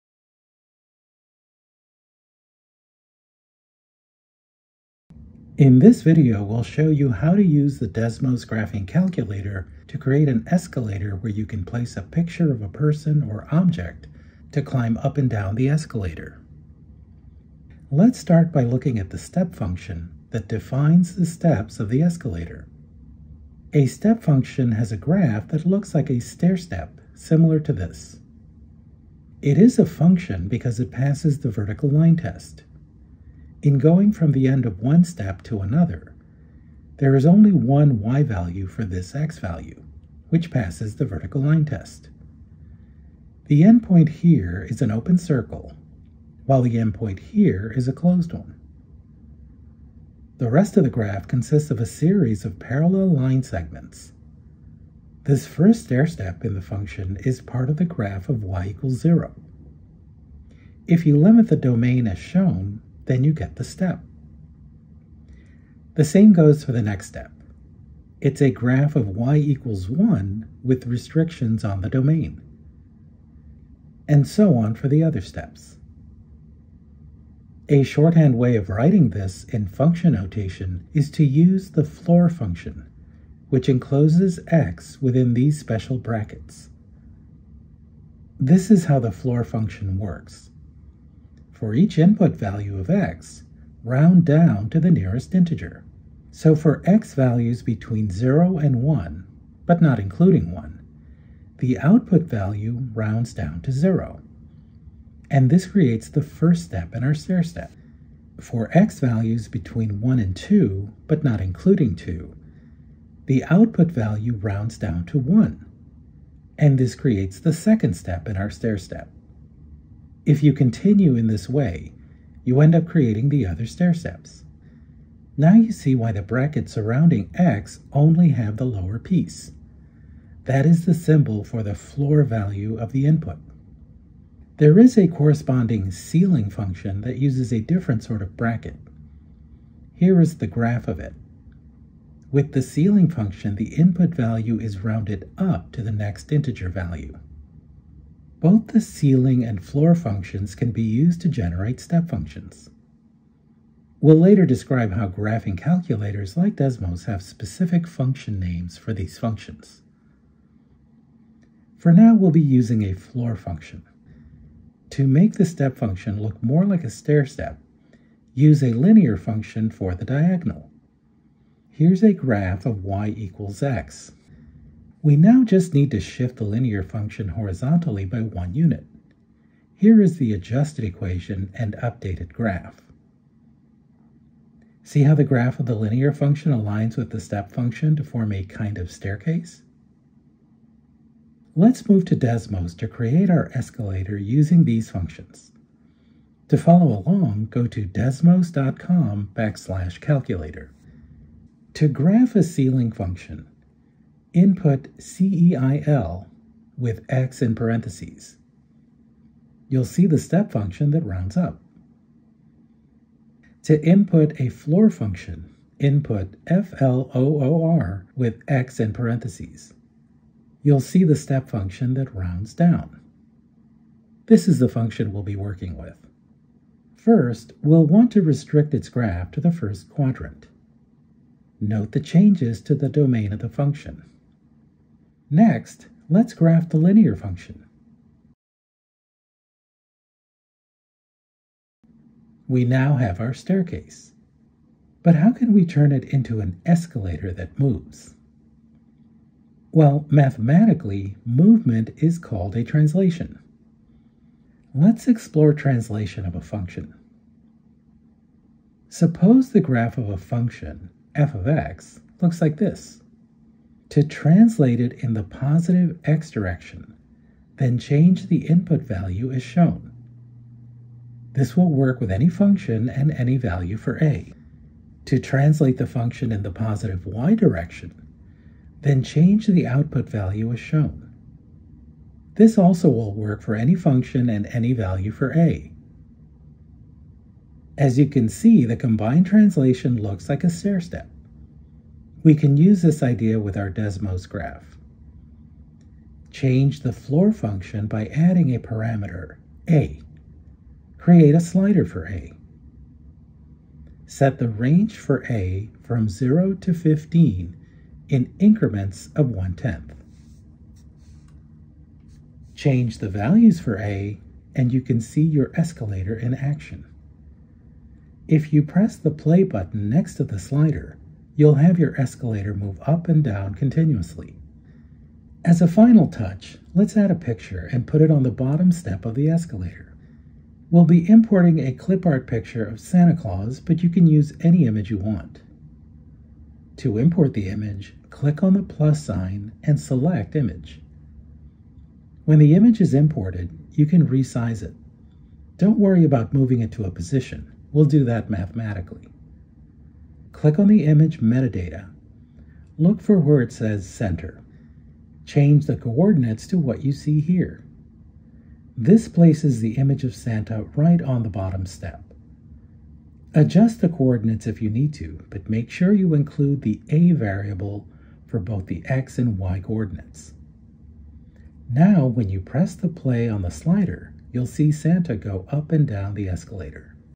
Desmos Video Tutorial: Step Functions
This video provides a step-by-step tutorial that shows how to graph a step function using the Desmos graphing calculator. This video also describes the properties of step functions and introduces the special functions used to generate the stair steps.

This video addresses these Common Core Standards: CCSS.MATH.CONTENT.HSF.BF.B.3, CCSS.MATH.CONTENT.HSF.IF.C.7.B